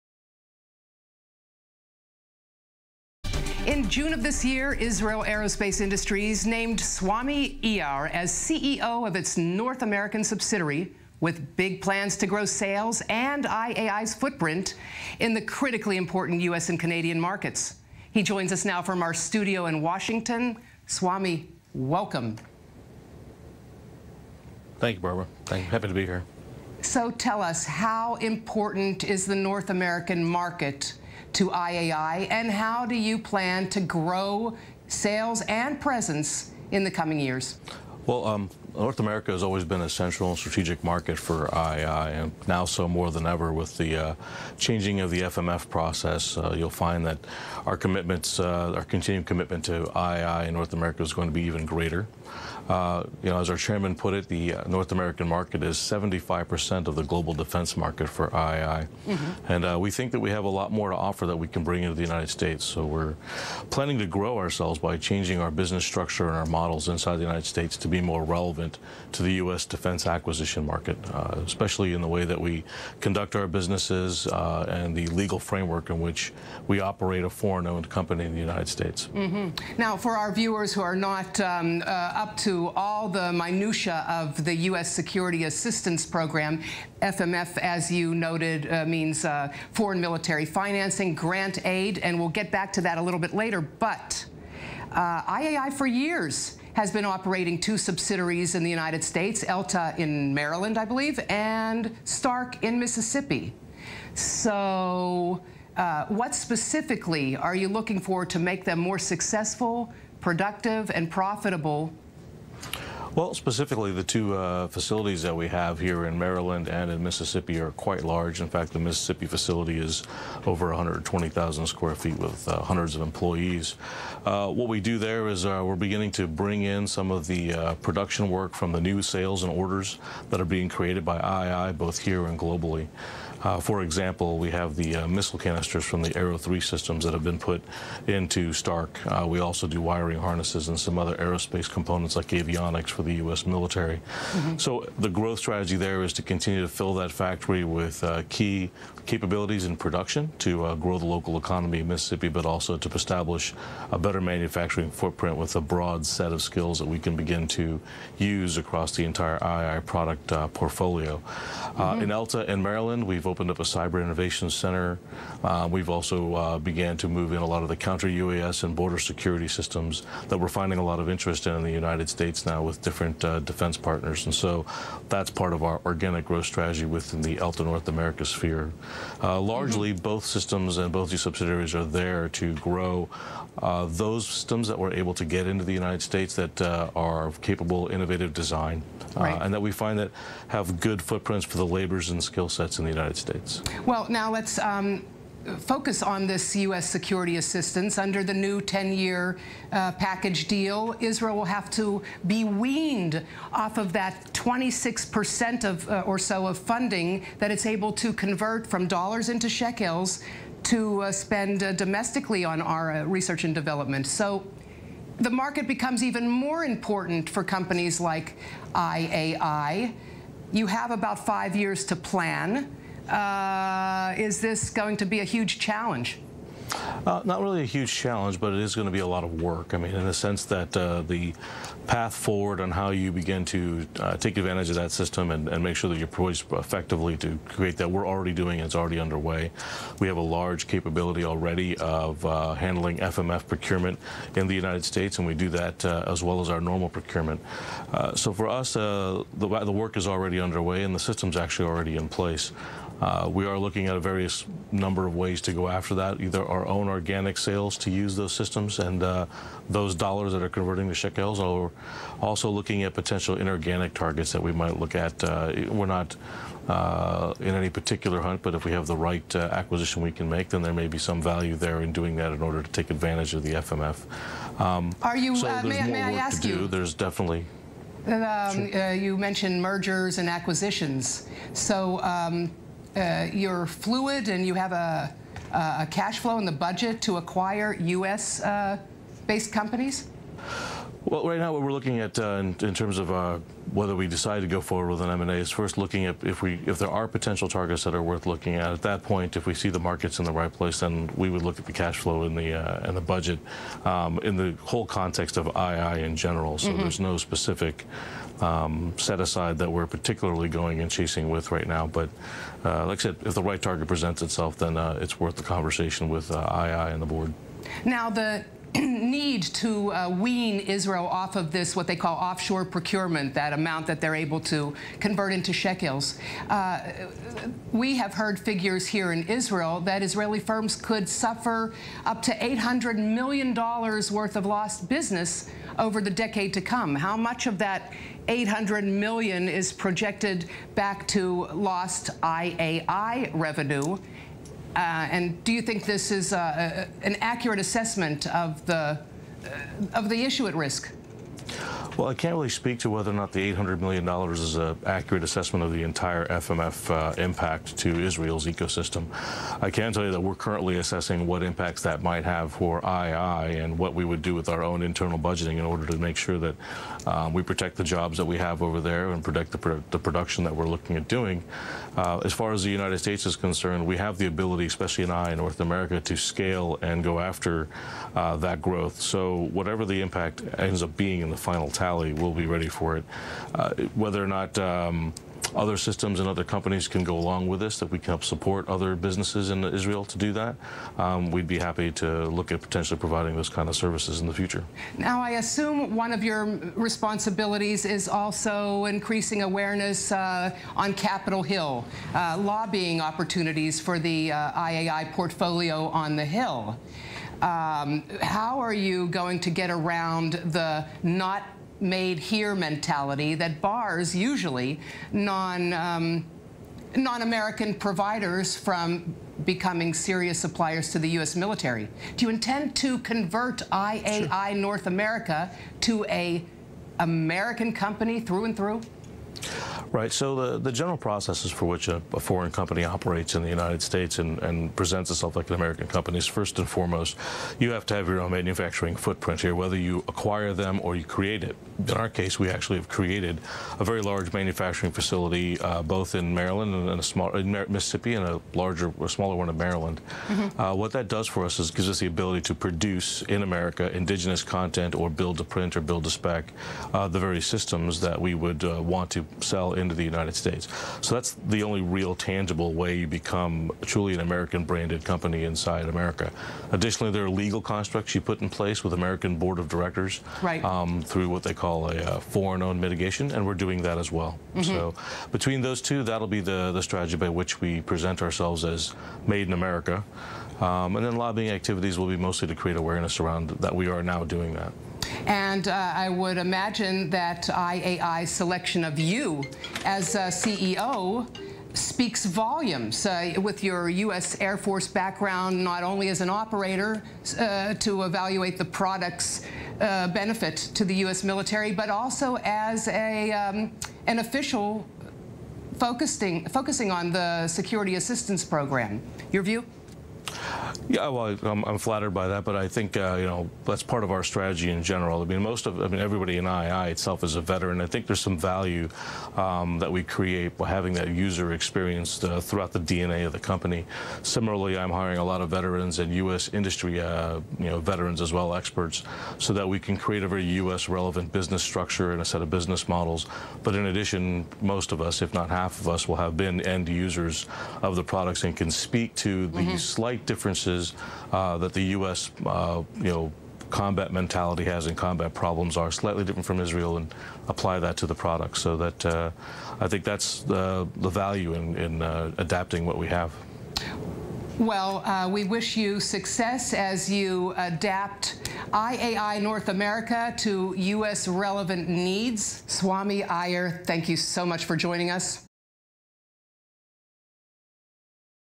One-on-One: Swami Iyer, CEO of Israel Aerospace Industries in D.C.
STRICTLY SECURITY | Named CEO of Israel Aerospace Industries in Washington D.C. Swami Iyar the company's North American subsidiary, with plans to grow sales and I.A.I's footprint in the crucial United States and Canadian markets. He joins Barbara Opall-Rome in a one-on-one interview.